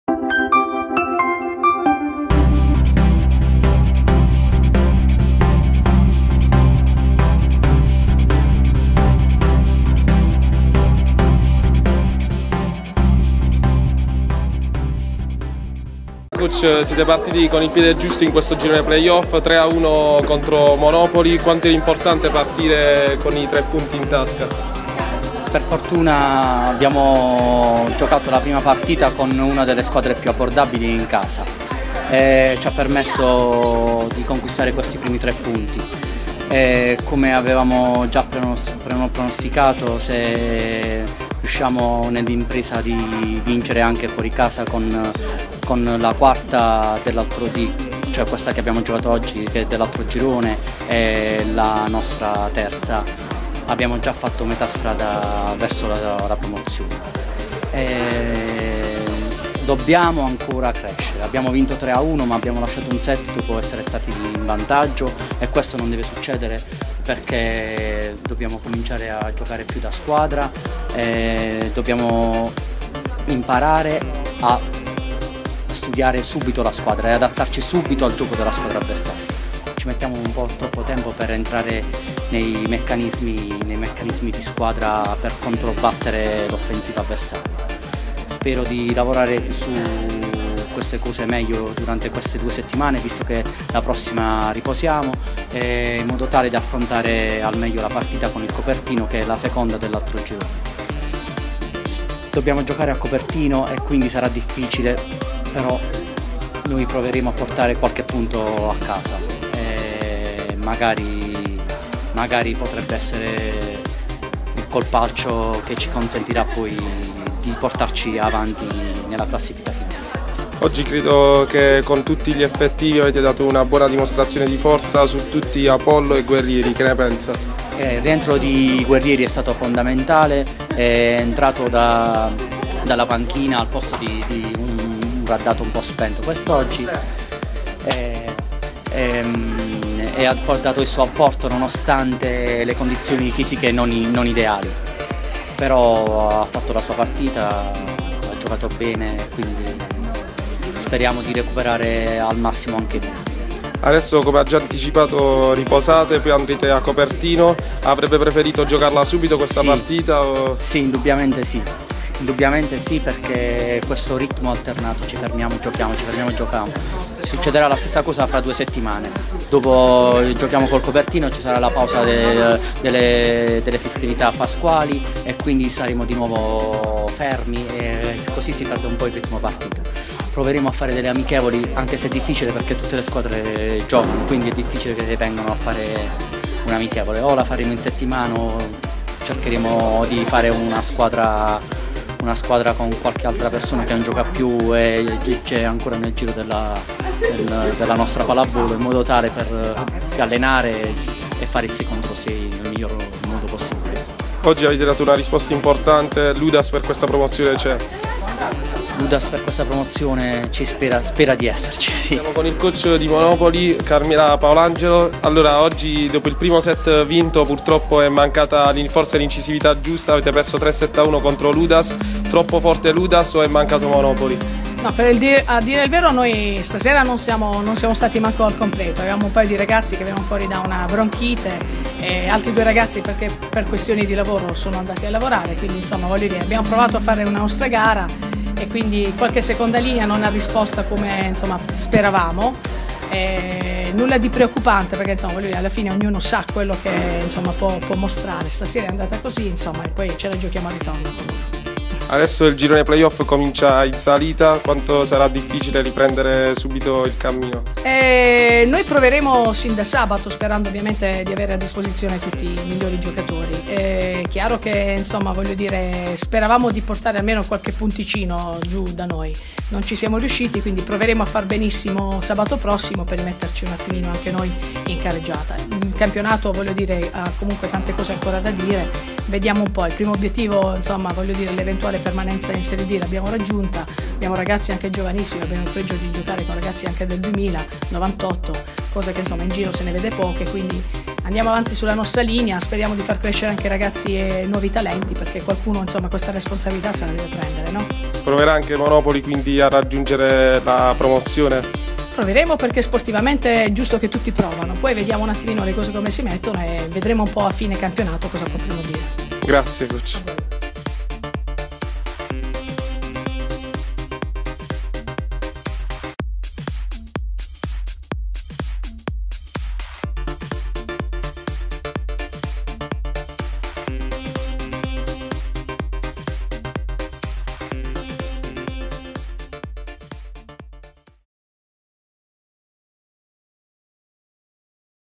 Udas Volley vs Monopoli Volley
Rivivi le emozioni dell'incontro di Campionato tra la Castellano Udas- Monopoli volley, incontro valido per i play-off  la serie D Regionaledi Volley .Inviato: Fabio Trallo Riprese e Montaggio: Tonya Gisario, a cura della redazione di Assist Sportivo